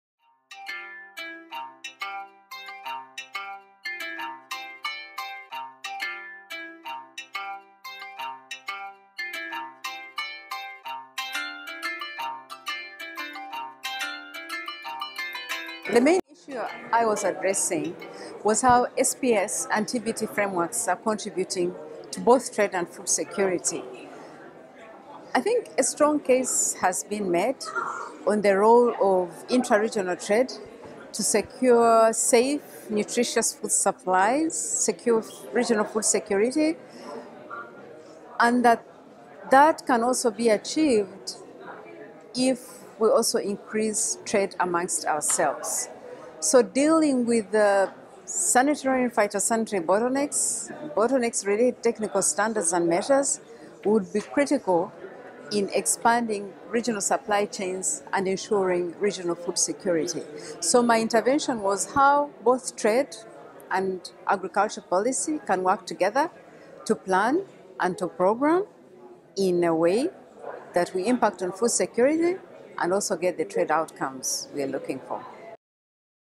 Martha Byanyima at the Platform Annual General Assembly
Martha Byanyima of the Common Market for Eastern and Southern Africa on regional trade’s contribution to food security